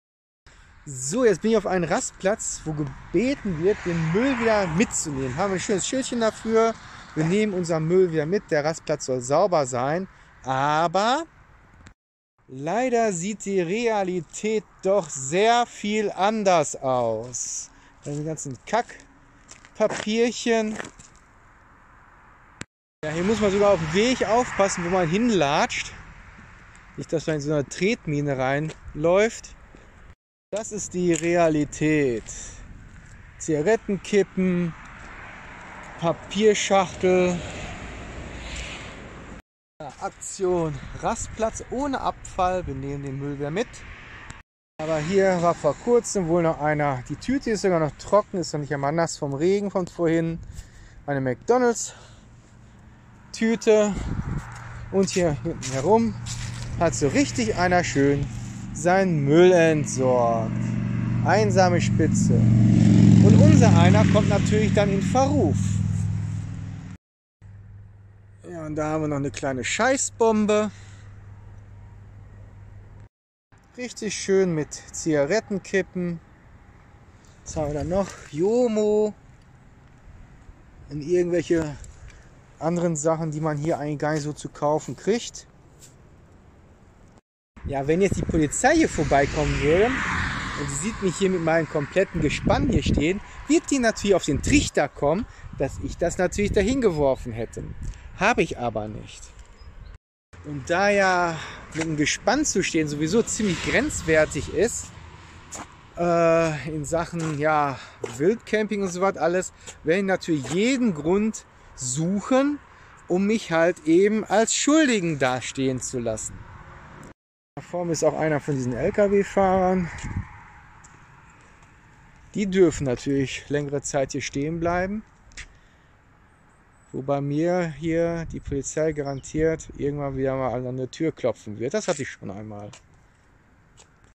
Müll auf einem Rastplatz
Müll auf einem Rastplatz wo gebeten wird ihn wieder mit zu nehmen doch was ich hier gesehen habe ist unverschämt und ein absolutes NO GO